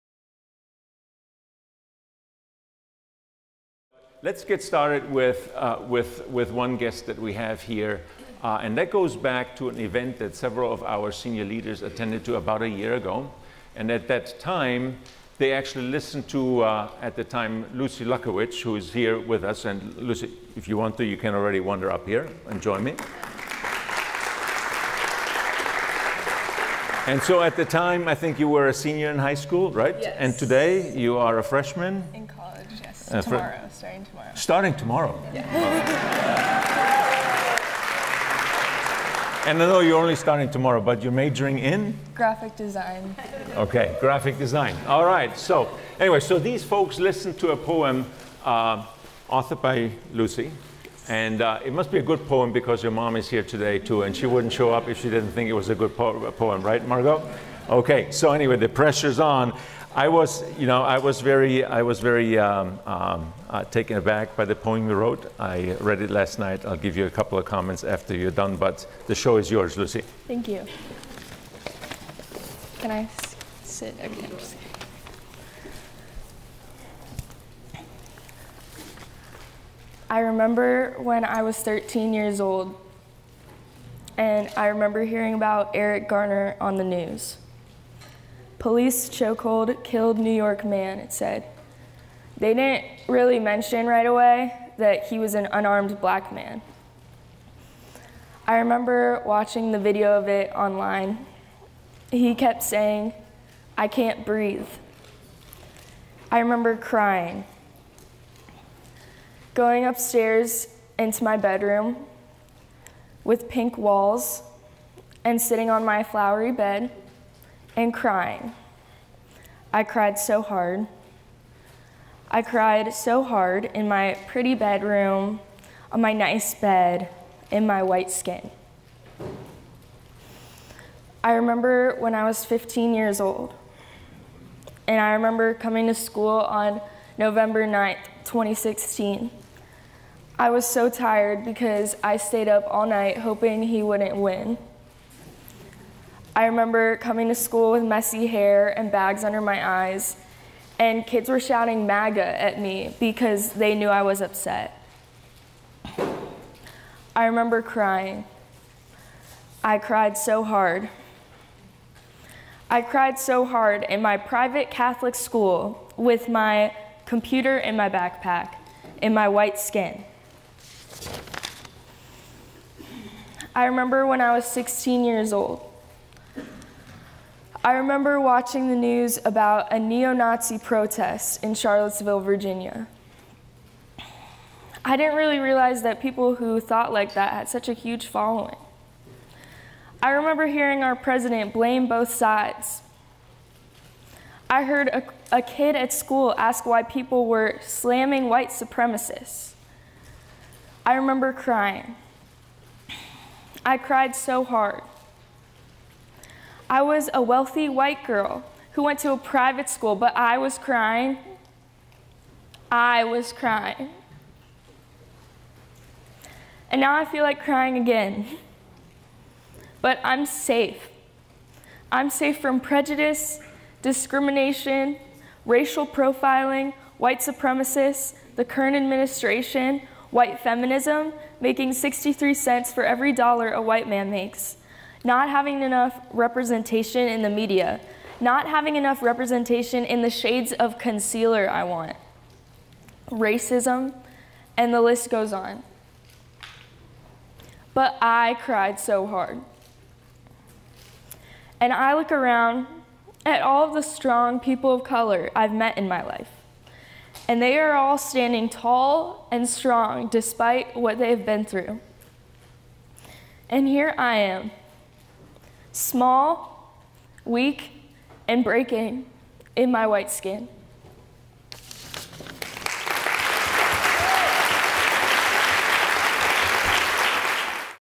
Starting the Conversation: Telling the Story of Our Common Humanity (Lucy Luckovich)
This Starting the Conversation series tells the story of our common humanity and includes an open conversation about race, class and gender.

Award-winning playwright, actor, producer & educator Fanshen Cox spoke with employees at Georgia-Pacific about implicit bias. Fanshen travels throughout the nation performing her show, which explores the complicated realities and perceptions of history, family, race, class, justice, and love.  The program also features employee testimonials.